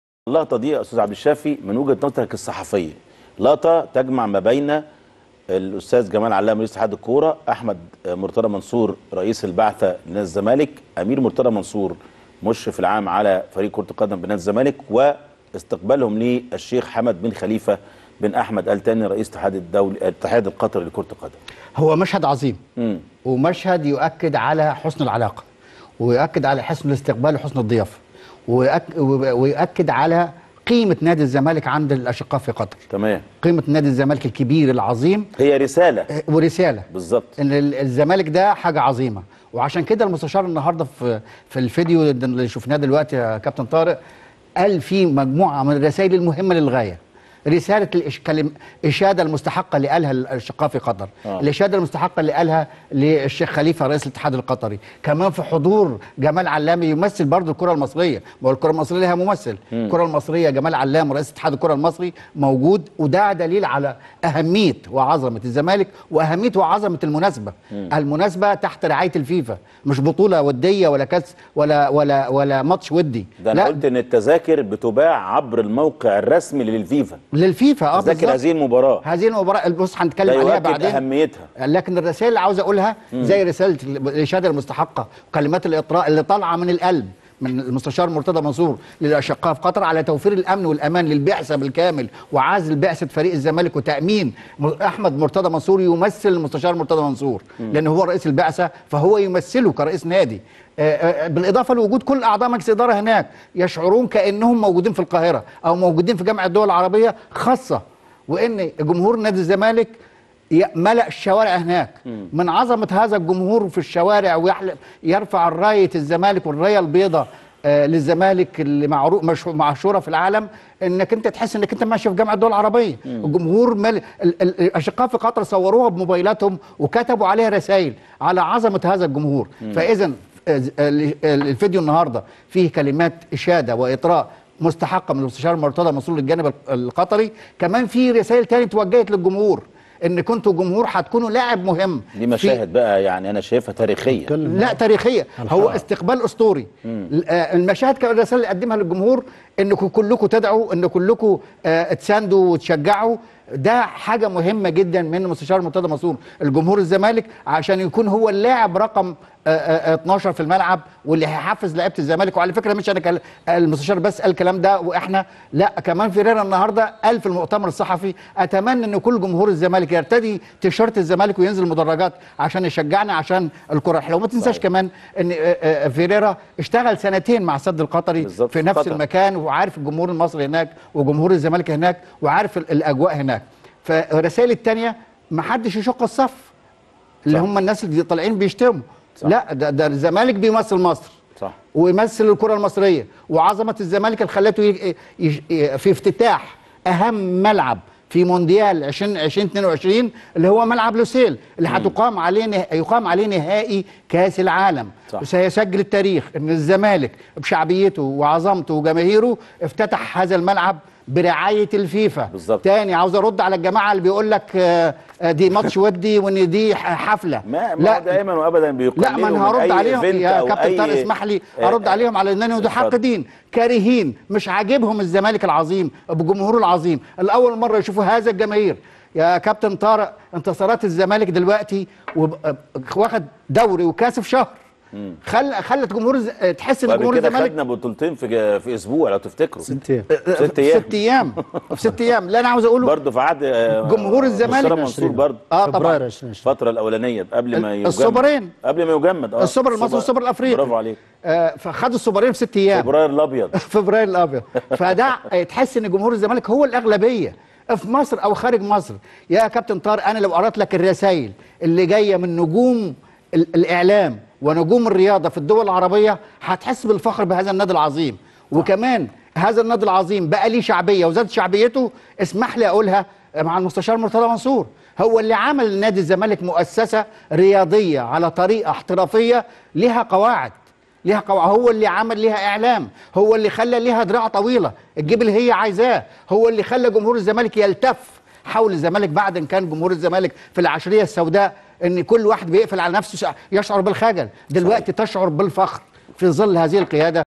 عبدالشافي صادق يعلق على استقبال أحمد مرتضى لـ الشيخ حمد بن خليفة بملعب لوسيل قبل مران الزمالك
عبدالشافي صادق يعلق على استقبال أحمد مرتضى لـ الشيخ حمد بن خليفة بملعب لوسيل قبل مران الزمالك - زملكاوي